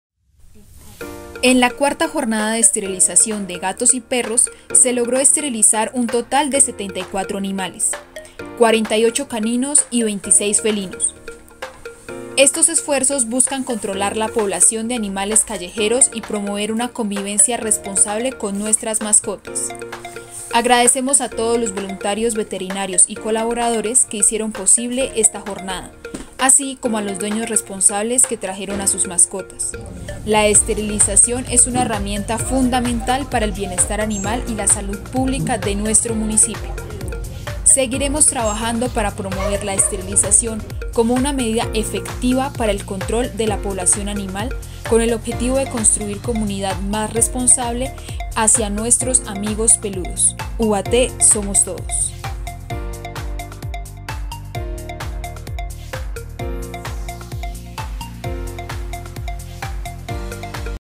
EL BIENESTAR ANIMAL EJE FUNDAMENTAL EN LA UBATÉ SOMOS TODOS
🐶🐱 Con éxito concluimos nuestra 4.ª Jornada de Esterilización canina y felina, interviniendo en total 74 animales. 💚

Desde la Dirección de Salud Pública seguimos apostando por esta medida para contribuir a la salud de las mascotas. Invitamos a todos los Ubatenses a participar en estos encuentros y considerar la adopción de mascotas.

¡Juntos construimos un entorno seguro y amoroso para estos animales! 🐾